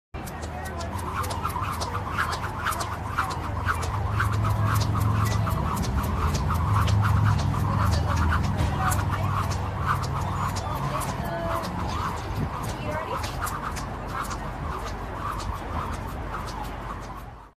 Model Kelly Gale works out in front of a fast food outlet
Victoria's Secret model Kelly Gale work out in front of a In 'N' Out Burger.